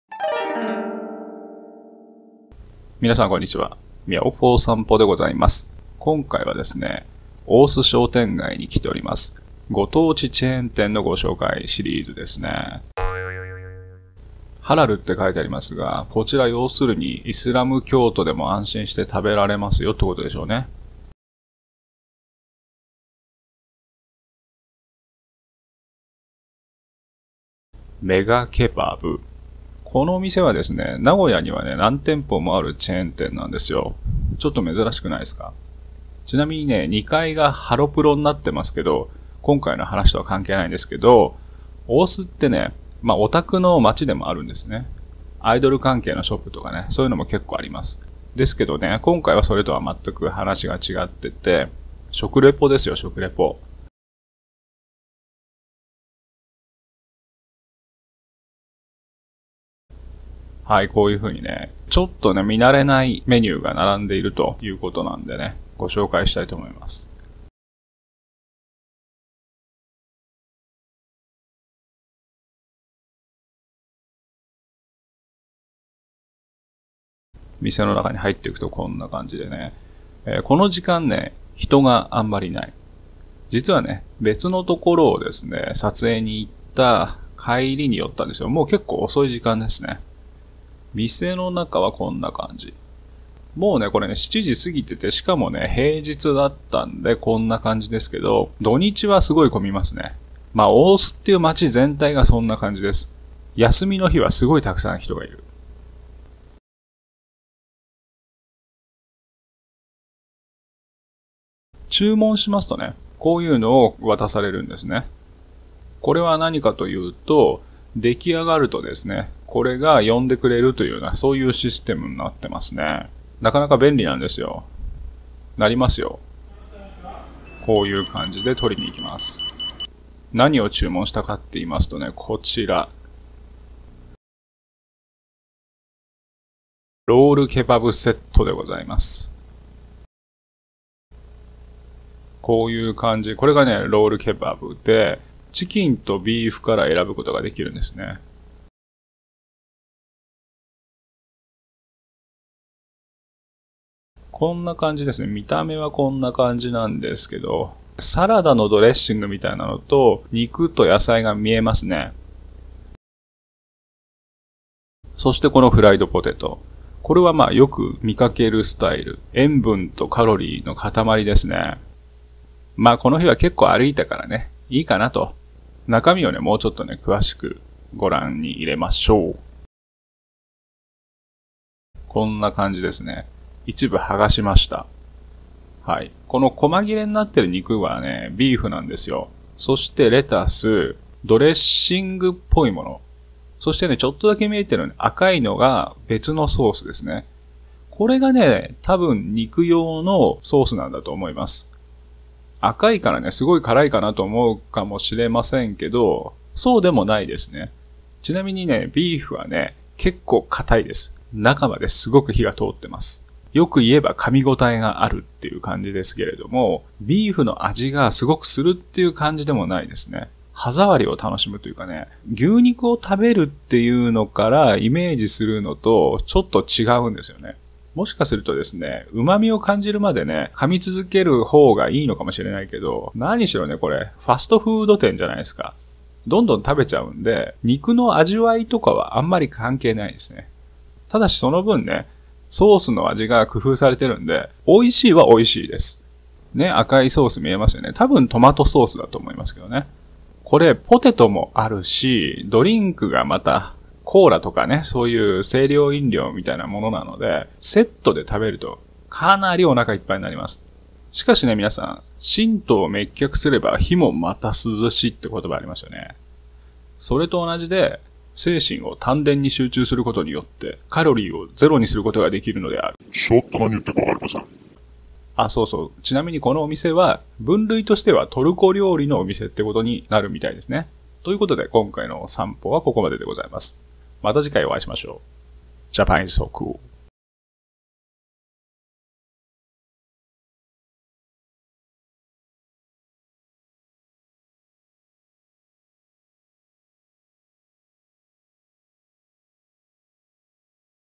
Turkey fast food in Nagoya/大須の「メガケバブ」でトルコ風ファストフードを食すの巻
心頭滅却すれば火もまた涼し！フライドポテトのカロリーも怖くないとゆーことでファストフード店でたらふく食べてしまいました。ハラルだし、たくさん歩いたし、大丈夫！

チャンネルのページ（登録お願いします！）